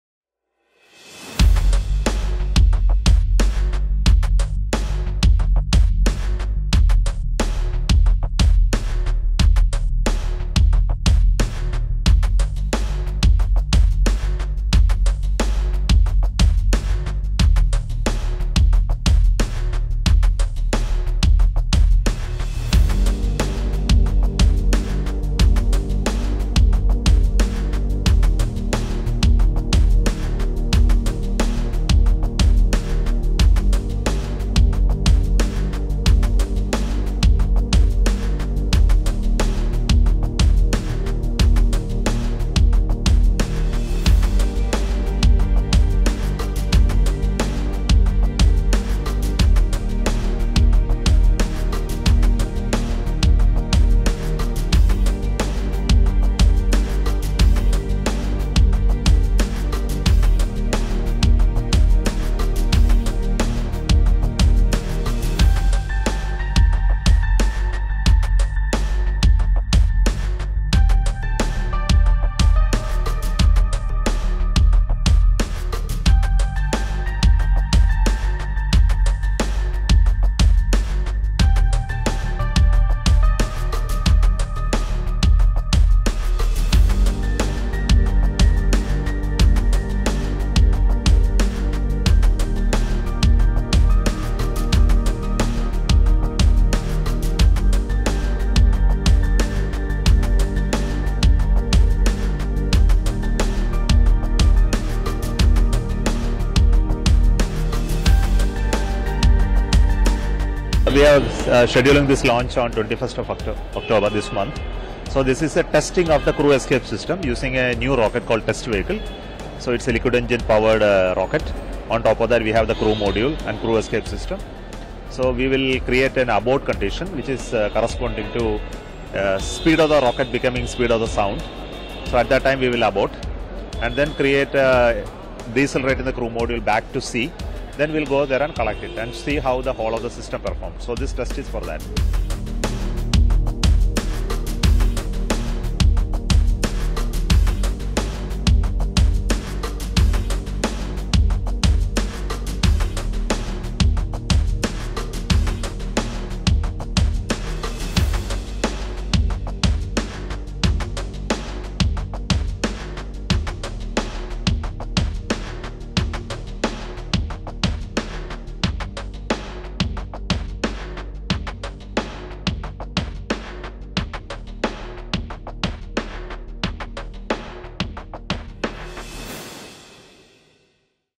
Isro's Gaganyaan test: Key details of the upcoming TV-D1 launch
The Indian Space Research Organisation (Isro) is preparing for a significant milestone in its ambitious Gaganyaan programme. On October 21 (Saturday), Isro plans to conduct the first development Flight Test Vehicle Abort Mission-1 (TV-D1), marking a crucial step towards India's first manned space mission. Watch the video to learn more.